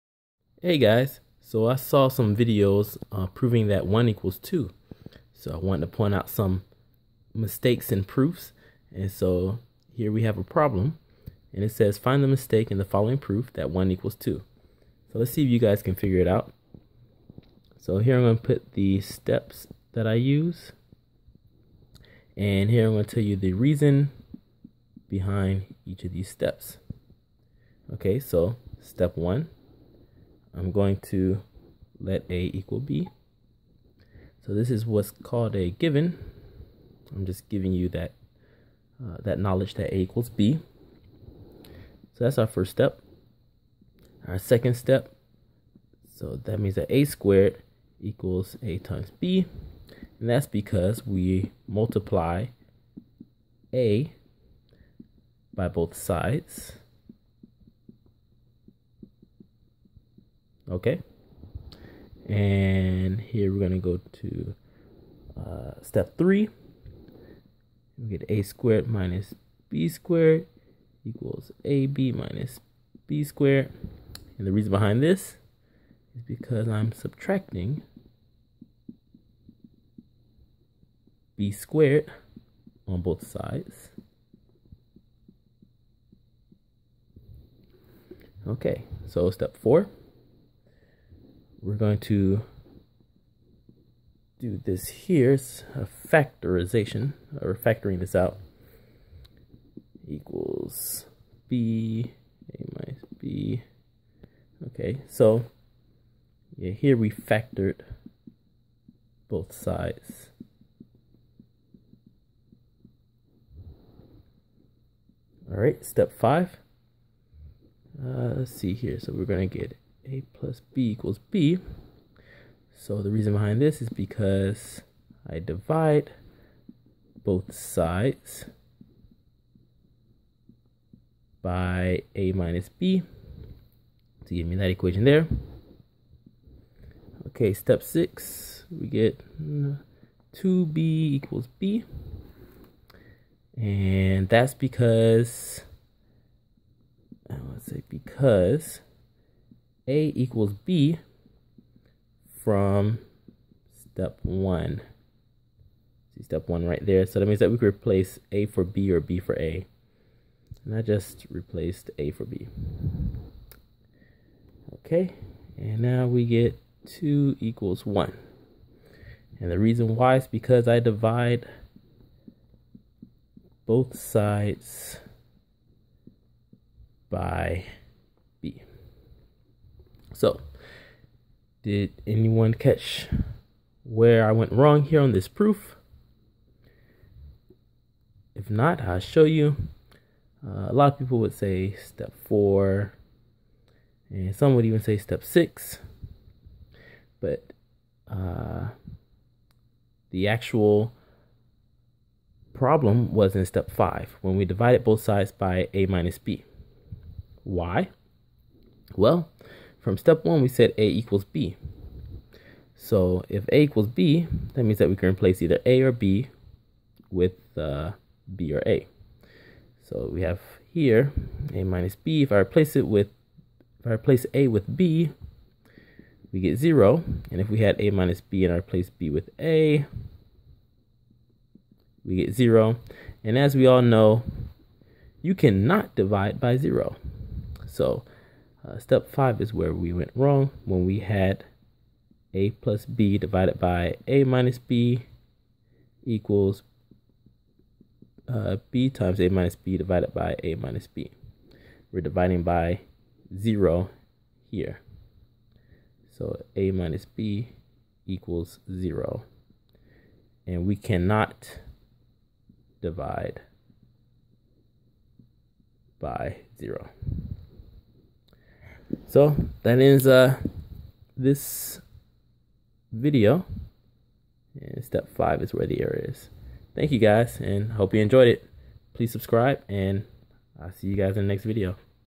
Mistakes in proofs
Learn one of the mistakes people can make in proofs.
Mistakes in the proof 2 =1